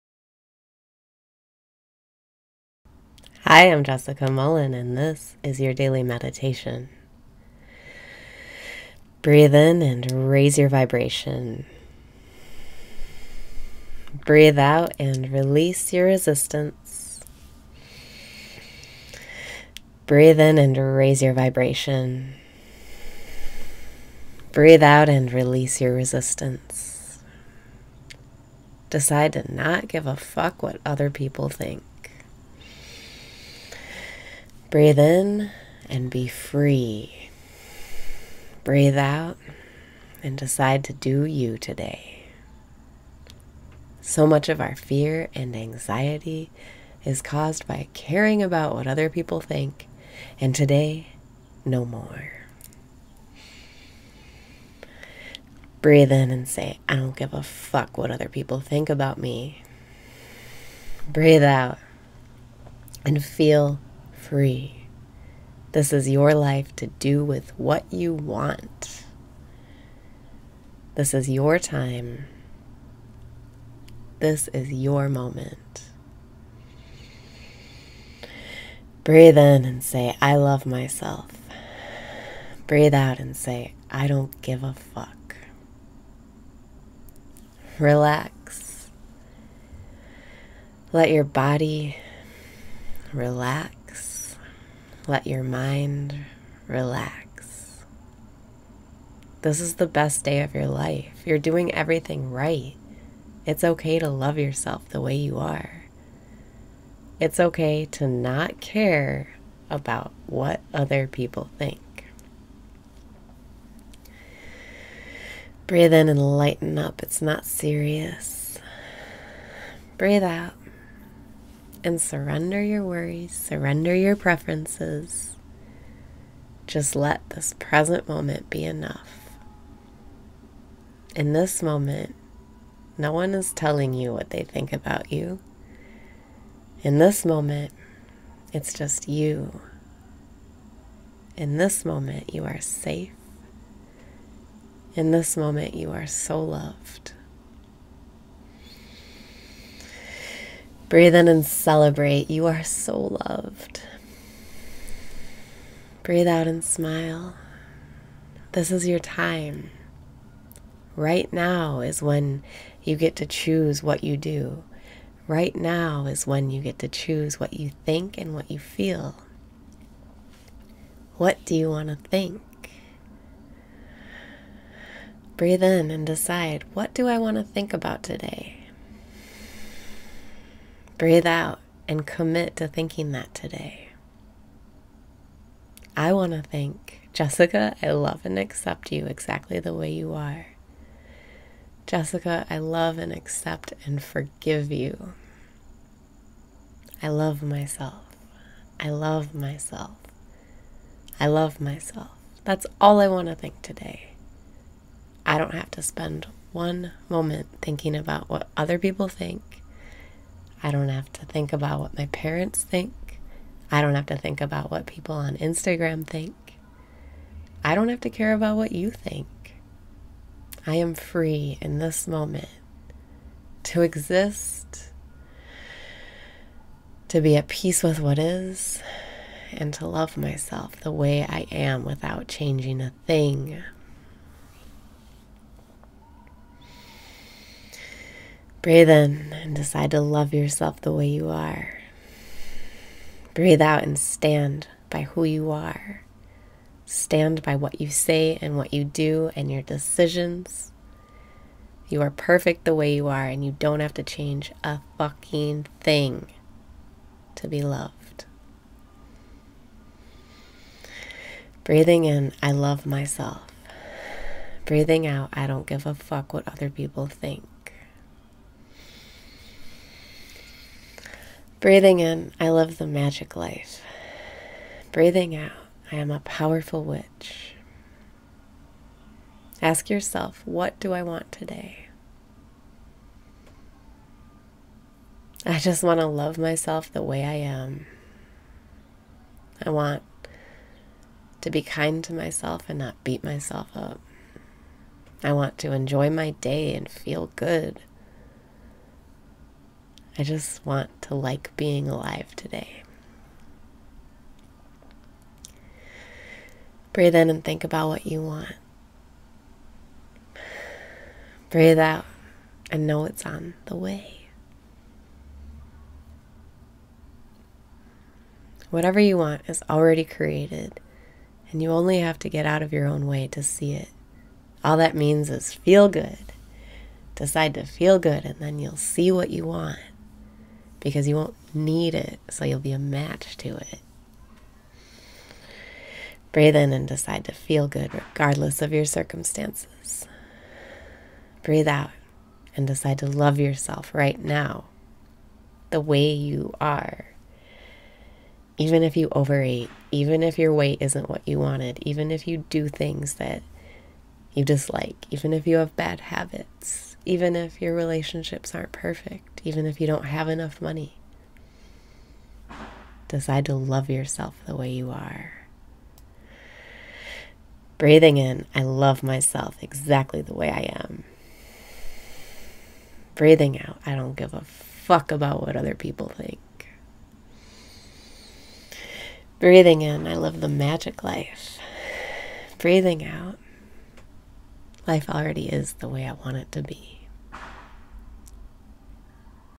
Daily Meditation - It's Ok to Not Care
Decide to not give a fuck what other people think!

Breathe in and be free!
Breathe out and decide to DO YOU today!

So much of our fear and anxiety is caused by caring about what other people think and today, no more!

Breathe in and say, “I don’t give a fuck what other people think about me!”
Breathe out and feel free!

This is your life to do with what you want! This is your time. This is your moment!

Relax. Let your body relax. Let your mind relax. This is the best day of your life! You’re doing everything right! It’s ok to love yourself the way you are! It’s ok to not care about what other people think!

Breathe out and surrender your worries! Surrender your preferences.

Just let this present moment be enough. In this moment, no one is telling you what they think about you. In this moment, it’s just you. In this moment, you are safe. In this moment, you are so loved.

Breathe in and celebrate! You are so loved!
Breathe out and smile! This is your time!

Right now is when you get to choose what you do. Right now is when you get to choose what you think and feel. What do you want to think?

Breathe in and decide, “What do I want to think about today?”
Breathe out and commit to thinking that.

I want to think, “Jessica, I love and accept you exactly the way you are. Jessica, I love and accept and forgive you. I love myself. I love myself. I love myself.” That’s all I want to think today. I don’t have to spend one moment thinking about what other people think. I don’t have to think about what my parents think. I don’t have to think about what people on instagram think. I don’t have to care about what you think. I am free in this moment to exist, to be at peace with what is, and to love myself the way I am without changing a thing!

Breathe in and decide to love yourself the way you are
Breathe out and stand by who you are

Stand by what you say and what you do and your decisions. You are perfect the way you are and you don’t have to change a fucking thing to be loved!

Breathing out I don’t give a fuck what other people think!

Breathing in I live the magick life!
Breathing out I am a powerful witch!

Ask yourself, “What do I want today?” I just want to love myself the way I am. I want to be kind to myself and not beat myself up. I want to enjoy my day and feel good. I just want to like being alive today.

Breathe out and know it’s on the way

Whatever you want is already created. You only have to get out of your own way to see it. All that means is feel good. Decide to feel good and then you’ll see what you want. Because you won’t need it, so you’ll be a match to it.

Breathe in and decide to feel good regardless of your circumstances
Breathe out and decide to love yourself right now the way you are!

Even if you overeat, even if your weight isn’t what you want, even if you do things you dislike, even if you have bad habits, even if your relationships aren’t perfect, even if you don’t have enough money, decide to love yourself the way you are.

Breathing in I love myself exactly the way I am
Breathing out I don’t give a fuck about what other people think

Breathing out life already is the way I want it to be!